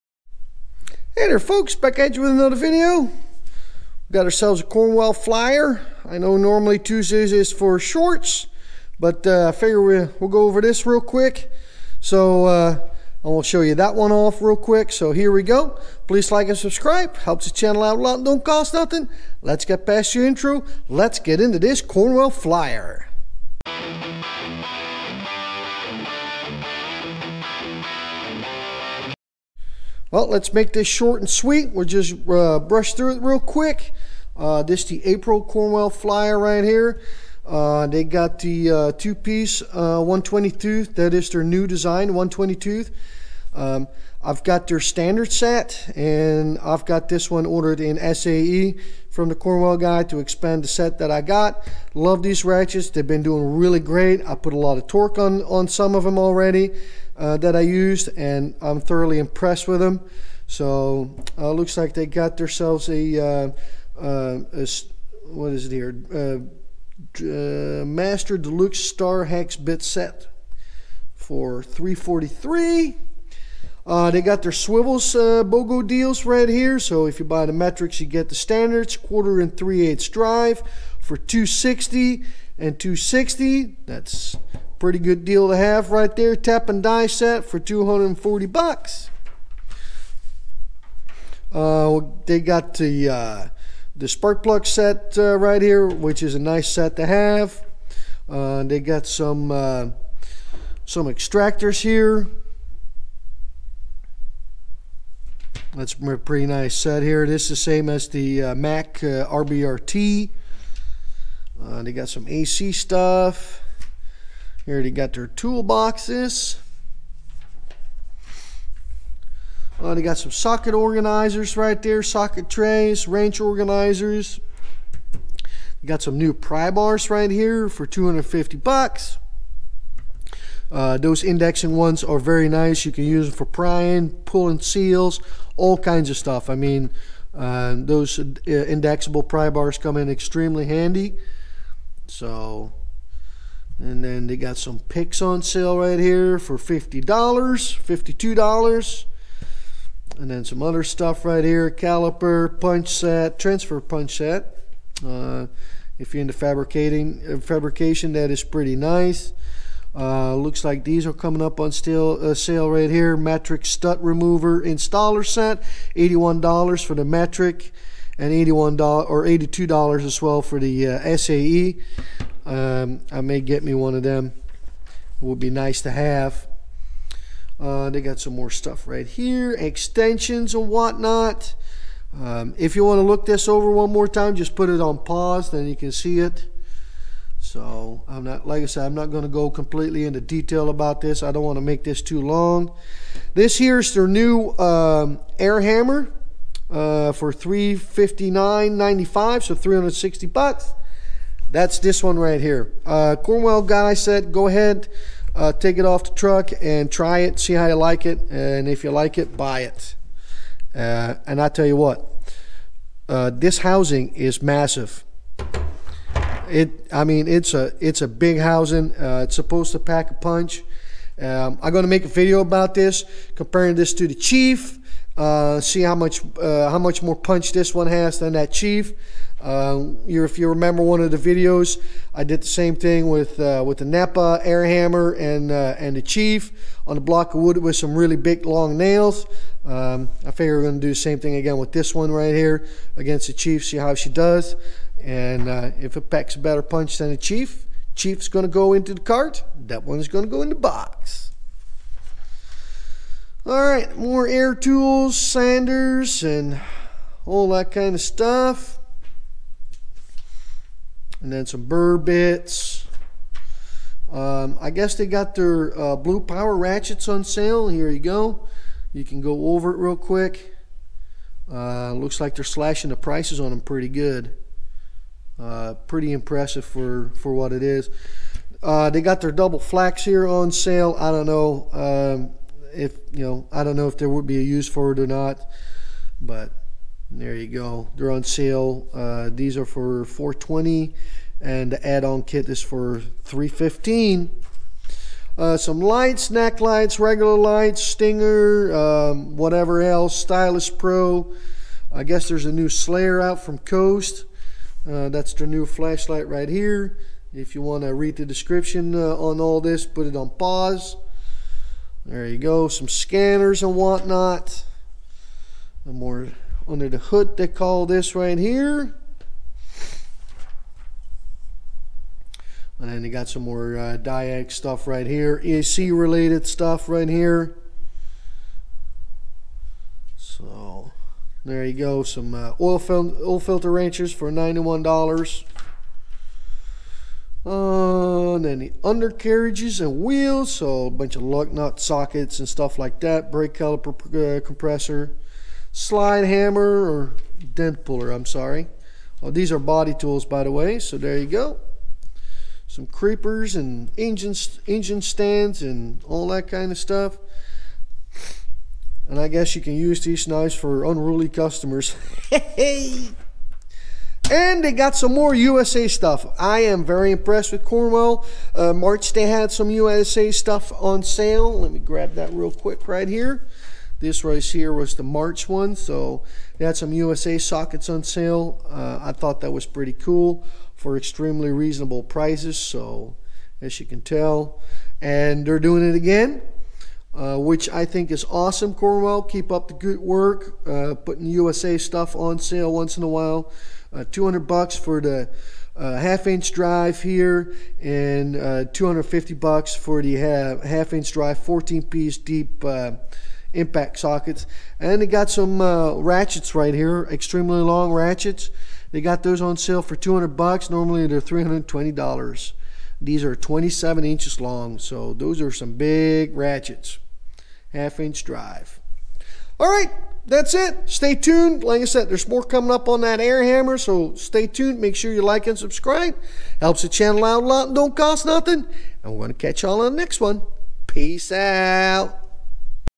Cornwell April flyer & more
@CornwellQualityTools April tool flyer and a little tool haul straight from the Cornwell tool truck. It's been awhile I did a tool flyer for you guys and we finally have one on time to be useful

So what do you think? Love it ? Hate it? Share your opinion.
Hey our Instagram has some cool things too
do you like to talk about tools ? Come check us out at our Facebook tool group